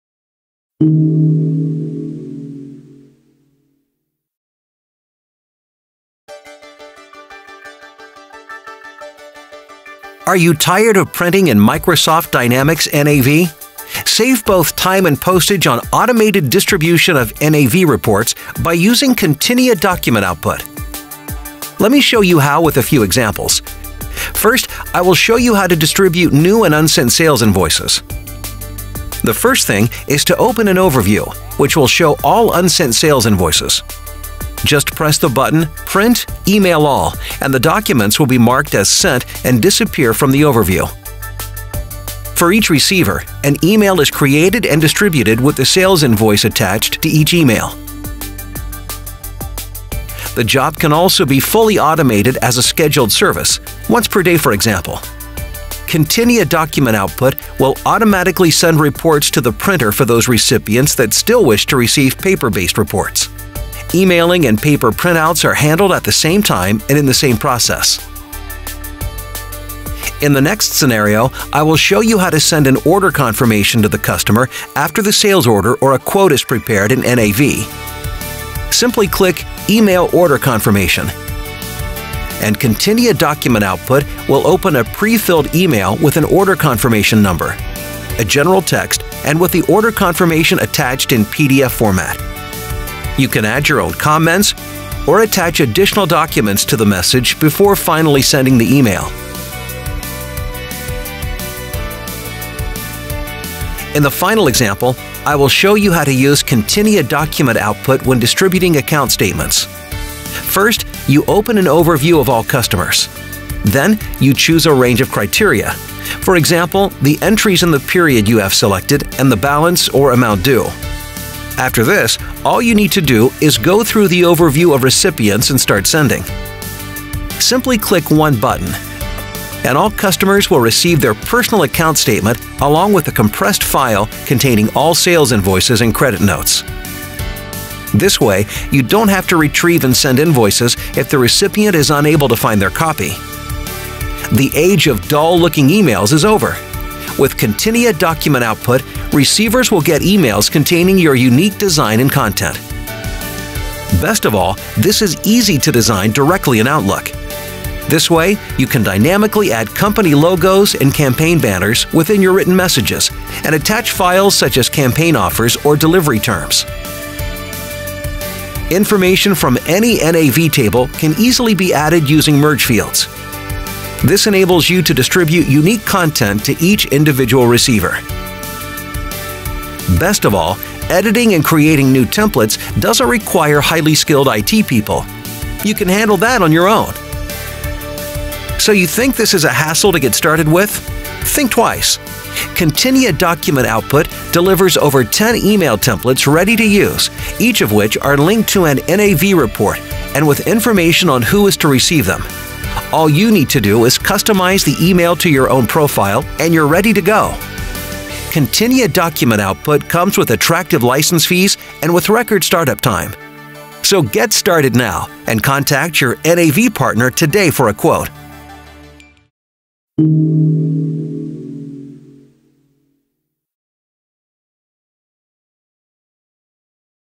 Continia Document Output for Microsoft Dynamics NAV
Automate the process of distributing documents and reports directly from Microsoft Dynamics NAV and save time and money on postage.